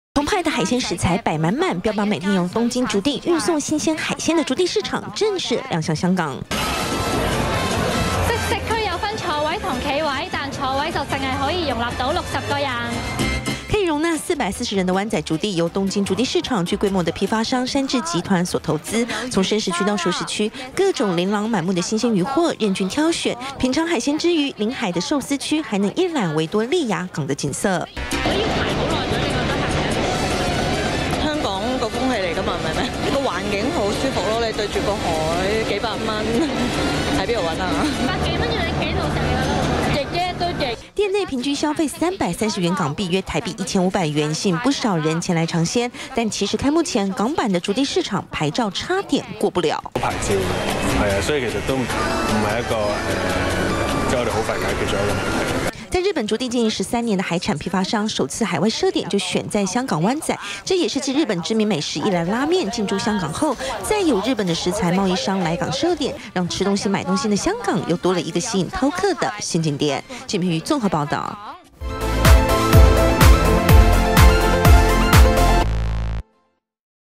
20161115中天新聞 香港築地市場開幕 漁貨標榜產地直送
日本東京知名的築地市場，總能吸引大批觀光客前去朝聖，而現在想吃築地海鮮，到香港旅遊也可以。在東京築地市場，經營多年的「山治集團」，旗下第一間海外水產市場，就選在香港灣仔開設分店，標榜所有食材，每天都從東京築地直送。